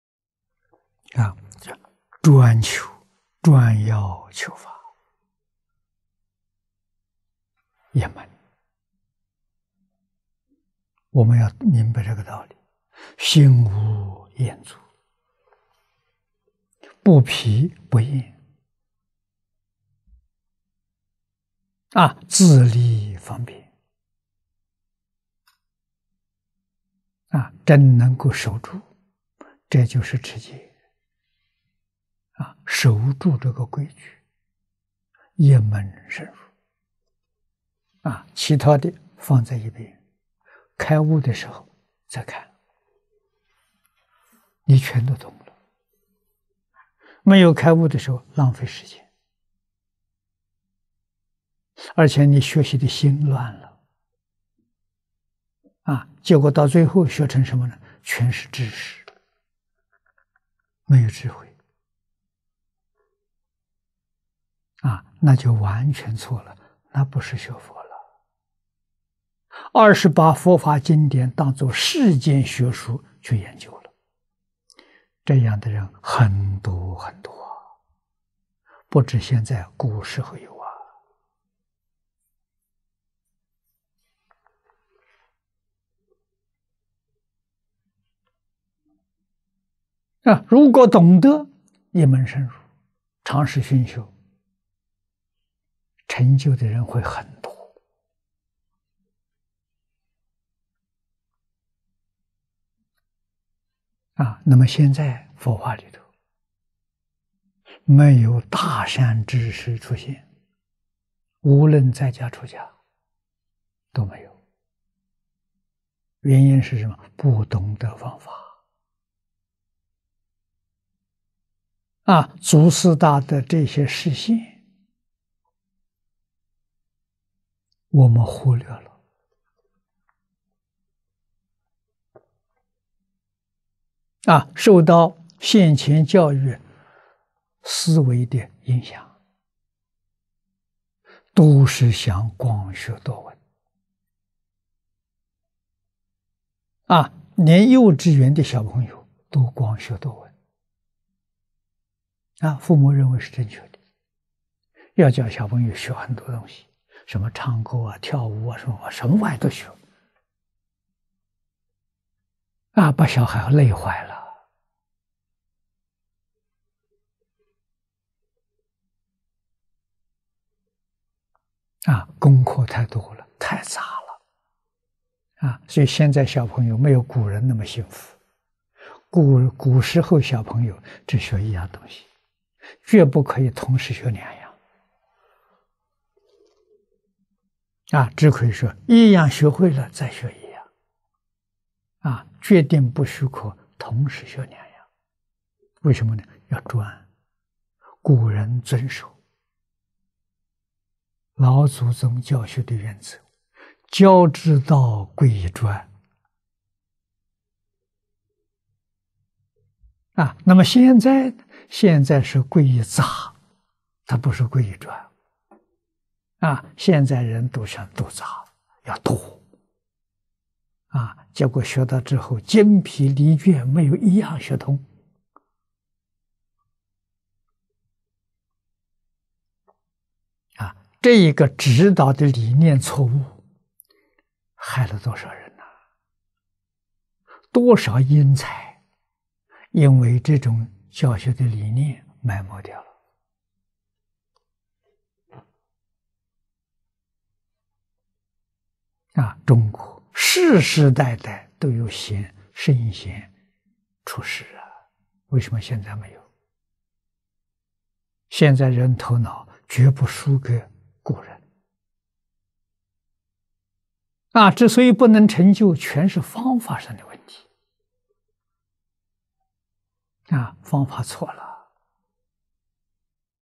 教之道，貴以專
專求，專樂求法，一門，我們要明白這個道理。心無厭足，不疲不厭，自利方便，真能夠守住，這就是持戒。守住這個規矩，一門深入，其他的放在一邊，開悟的時候再看，你全都懂了。沒有開悟的時候浪費時間，而且你學習的心亂了，結果到最後學成什麼？全是知識，沒有智慧，那就完全錯了。那不是學佛，而是把佛法經典當作世間學術去研究，這樣的人很多很多，不只現在，古時候有。如果懂得一門深入，長時薰修，成就的人會很多。現在佛法裡頭沒有大善知識出現，無論在家、出家都沒有。原因是什麼？不懂得方法，祖師大德這些示現我們忽略了。受到現前教育思惟的影響，都是想廣學多聞，連幼稚園的小朋友都廣學多聞，父母認為是正確的。要教小朋友學很多東西，什麼唱歌、跳舞，什麼玩意都學，把小孩累壞了，功課太多、太雜了。所以，現在小朋友沒有古人那麼幸福，古時候小朋友只學一樣東西，絕不可以同時學兩樣。只可以說一樣學會了再學一樣，決定不許可同時學兩樣。為什麼？要專，古人遵守老祖宗教學的原則，「教之道，貴以專」。那麼現在，現在是貴以雜，他不是貴以專。現在人都想多學，要多，結果學到之後精疲力倦，沒有一樣學通。這個指導的理念錯誤害了多少人？多少英才因為這種教學的理念埋沒掉了。
　　中國世世代代都有聖賢出世，為什麼現在沒有？現在人頭腦絕不輸給古人，之所以不能成就，全是方法上的問題，方法錯了。

文摘恭錄—淨土大經解演義（第四八七集）2011/7/9　檔名：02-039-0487